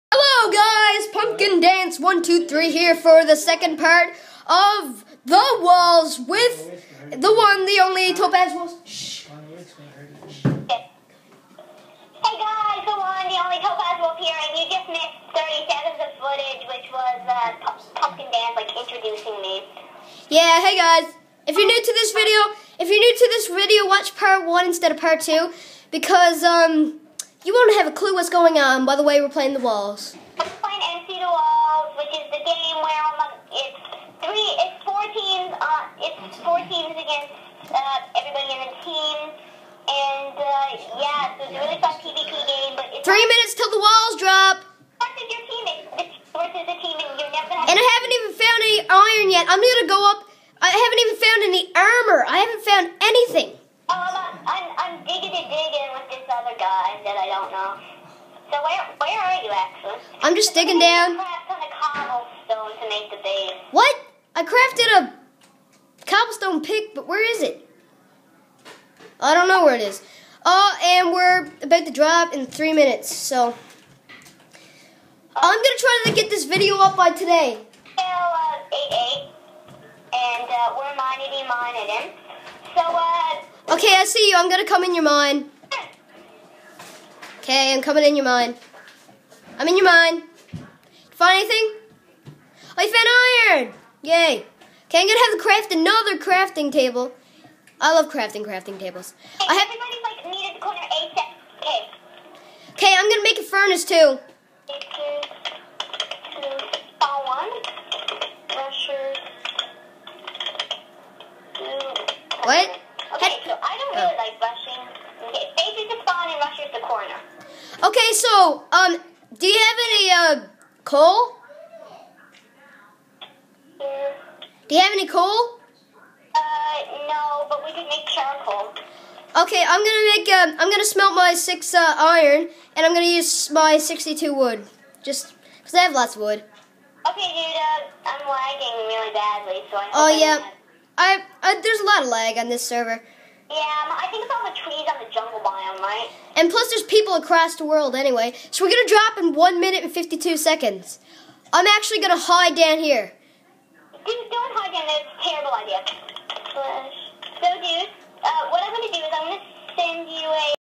The walls part 2
Watch me and topaz battle in the walls!

Be sure to check out topazes channel!

Bye!